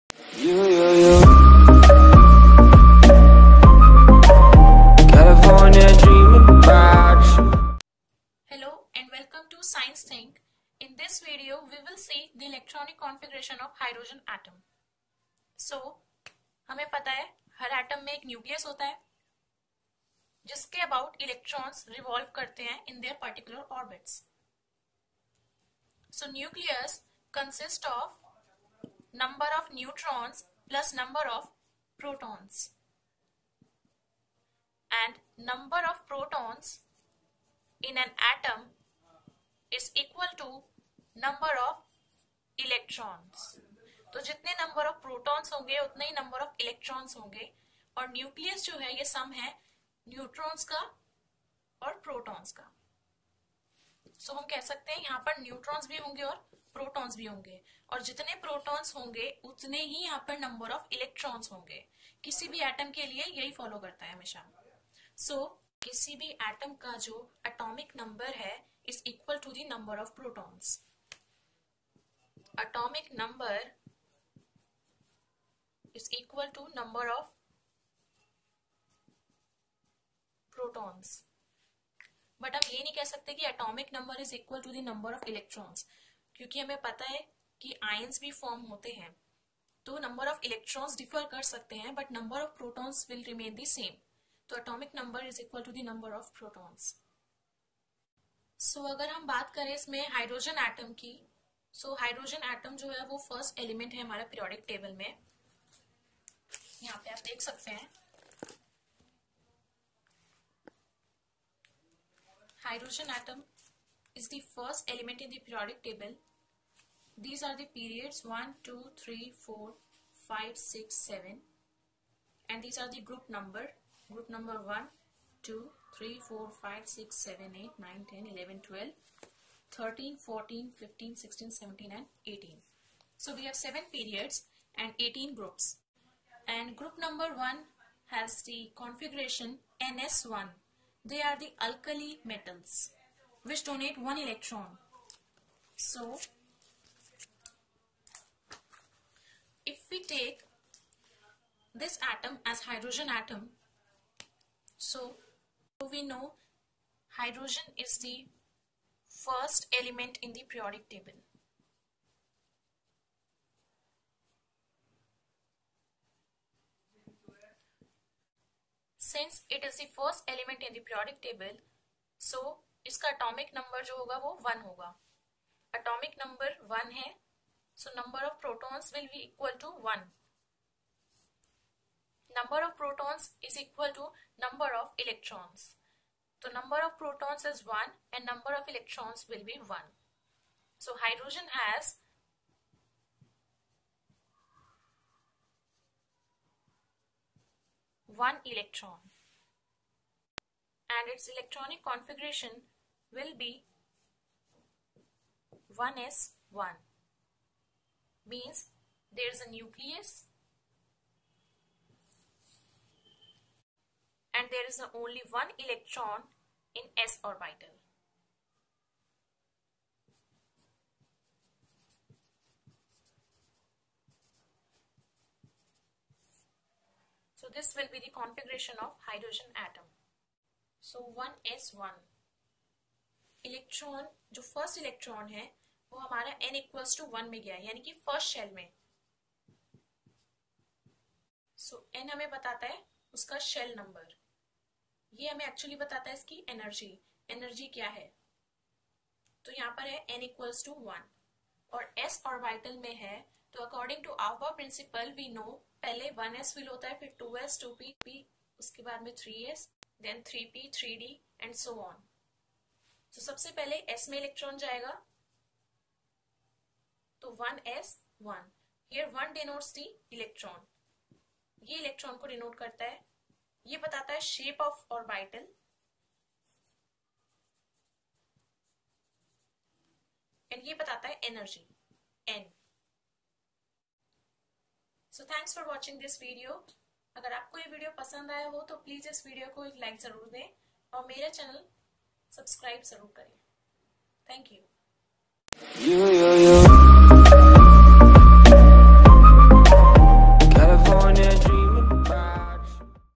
Electronic Configuration Of Hydrogen Atom- SCIENCE THINK
Hello
Welcome to "SCIENCE THINK".

This Video is about the electronic Configuration Of Hydrogen . Watch this video till end . Hope you like it !!!